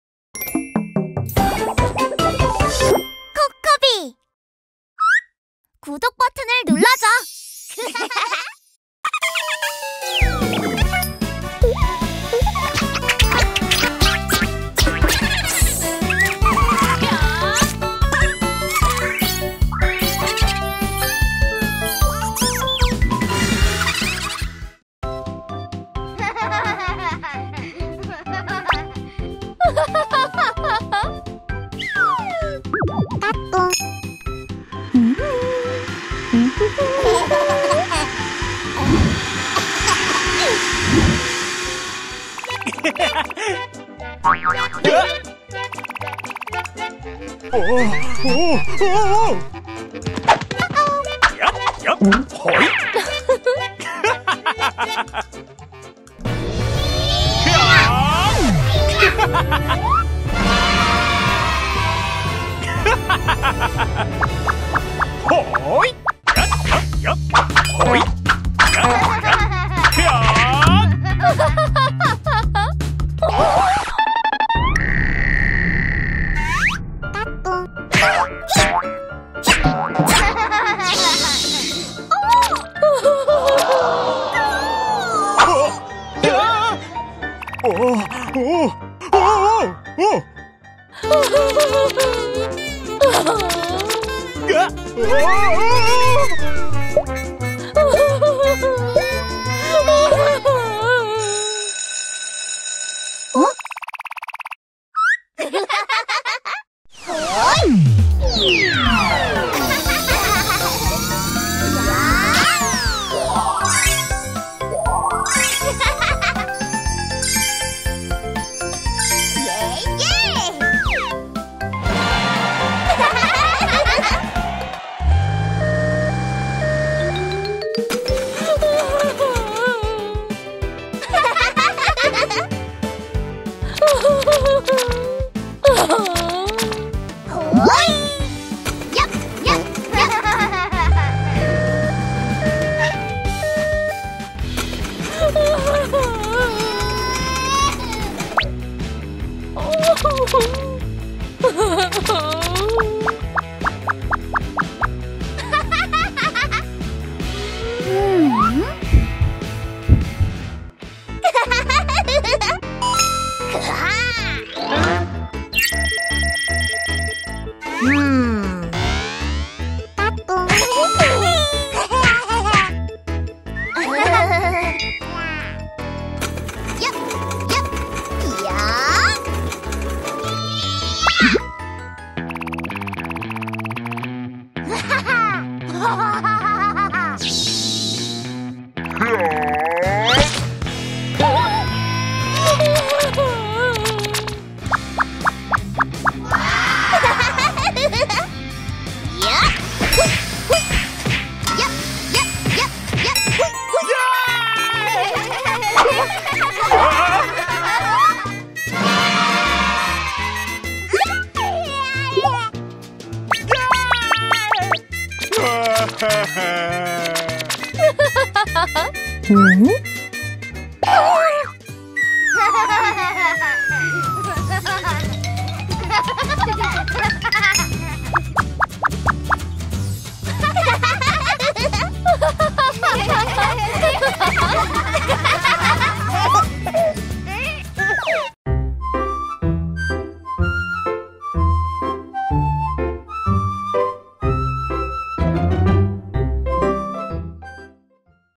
아기가 태어나요! 엄마를 도와 병원으로 가요 | 코코비 장난감 병원놀이
의사선생님, 의사, 치료